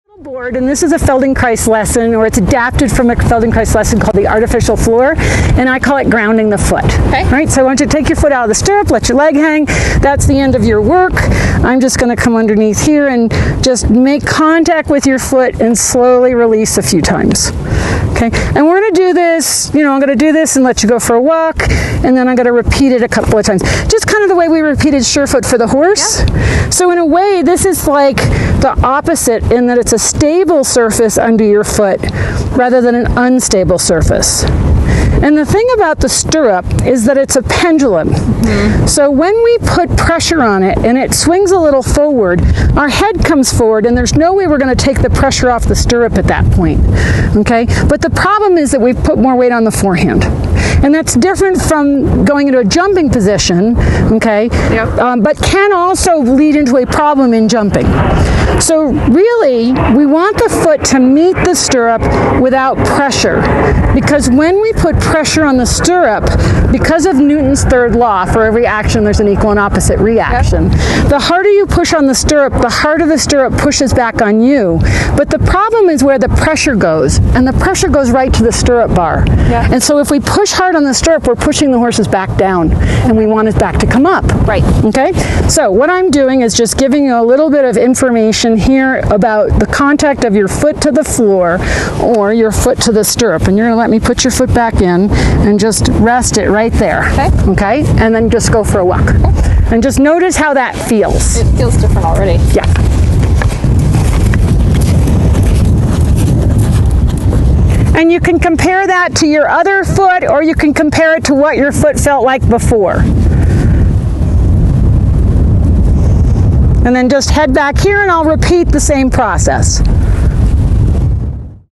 Grounding the Foot with Wendy Murdoch
Enjoy a preview of this lesson!

Without knowing or being aware, many riders push down on their stirrups. This force of is translated into the horse’s back, the very thing we are tying to lift with good riding. In this segment Wendy Murdoch demonstrates a quick and simple way to help a rider release this pressure creating freer movement in horse and rider.

The full length of this lesson is 8:51